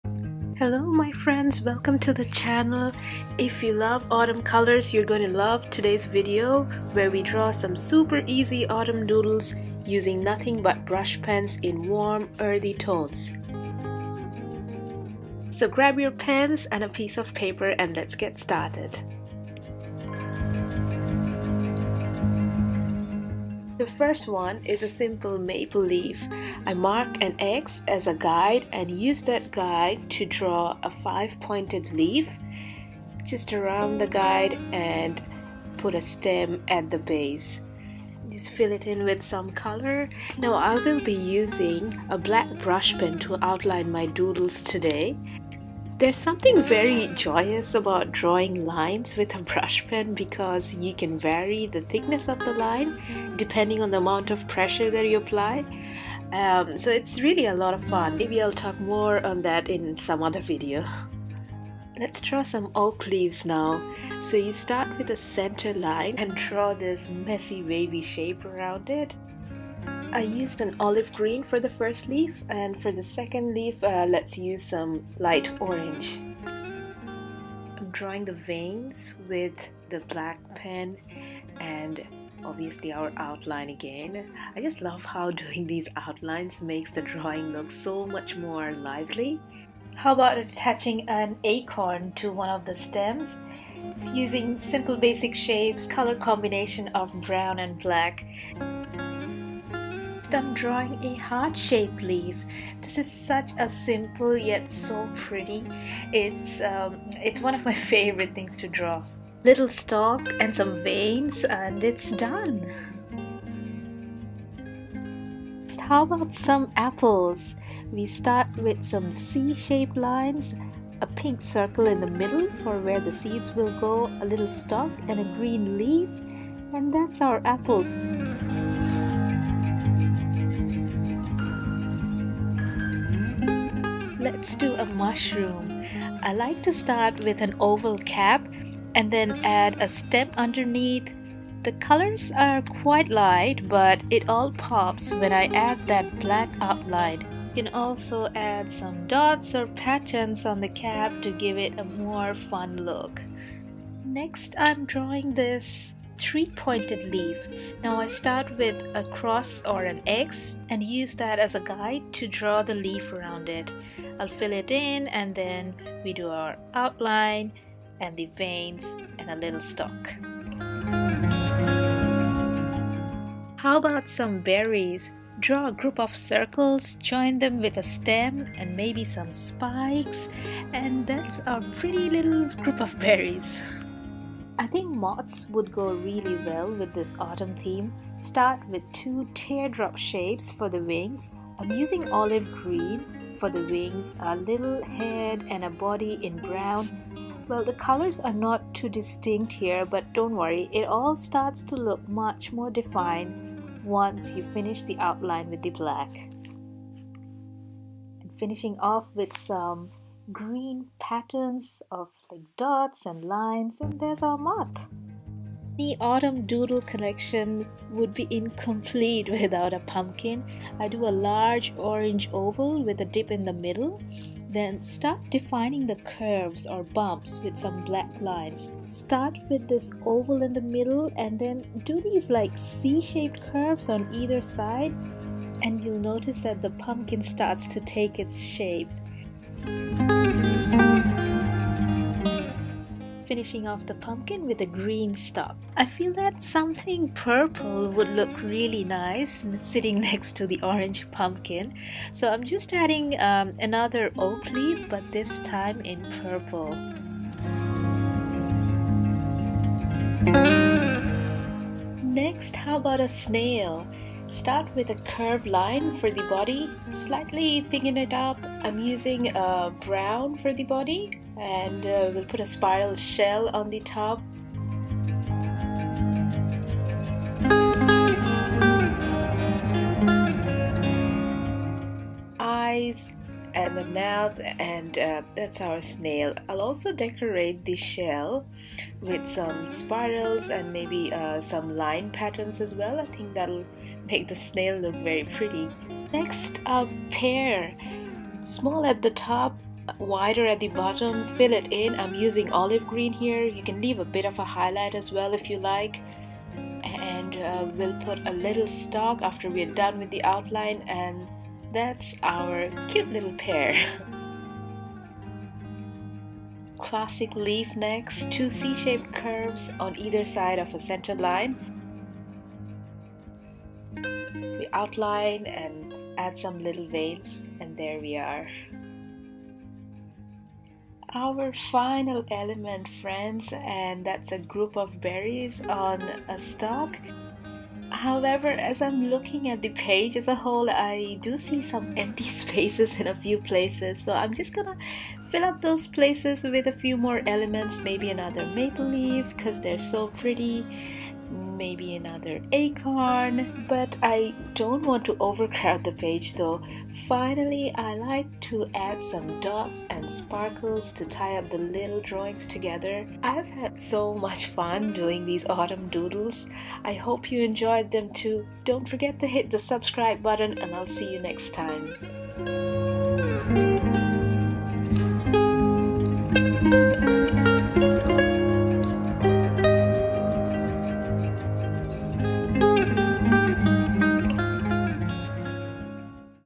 Autumn Brushpen Doodles anyone can draw | Cute Autumn Doodle Ideas with how to draw tutorial
Get some inspiration for your fall bullet journal, sketchbook and more with these simple yet fun autumn/ fall doodles. In this video I show you how to draw these cute autumn doodles in simple easy steps. I hope you enjoy the video!